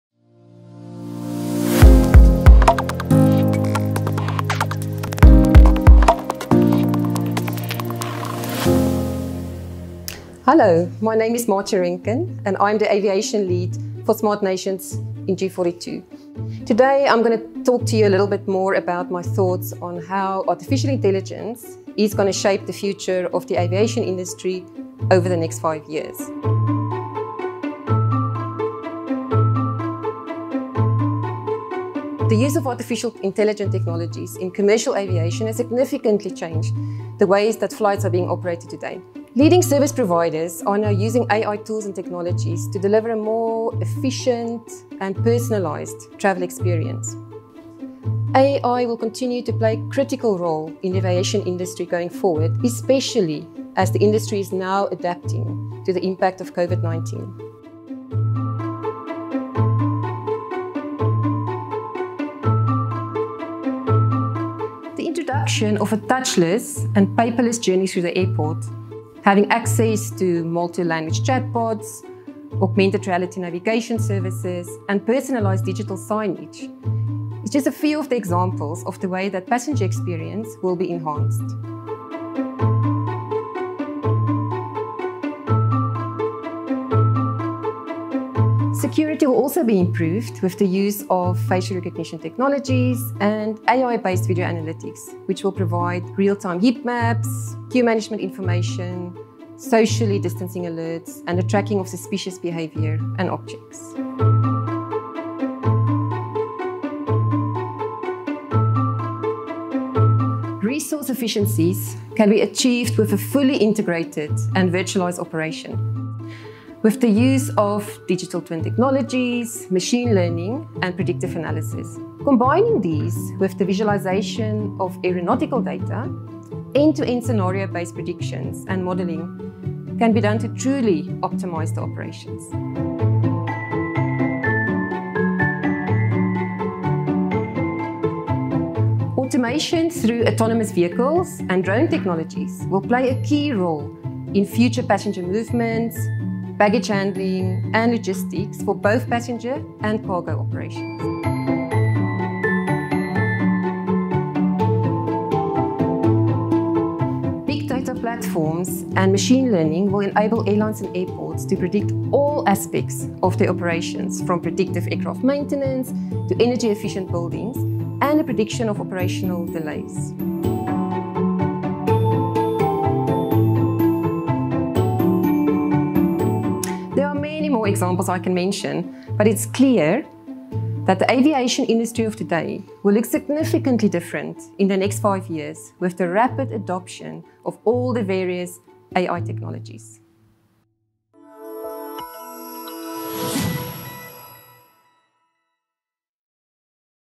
AI Answers: Our G42 Aviation Lead discusses how AI can add value to Aviation
AI is redefining the way major industries across the world operate.

In our new series AIanswers, G42 experts talk about how AI can add value to multiple industries. In today’s episode Maatje Rencken, Aviation Lead at G42SmartNation talks about AI and Aviation ✈️.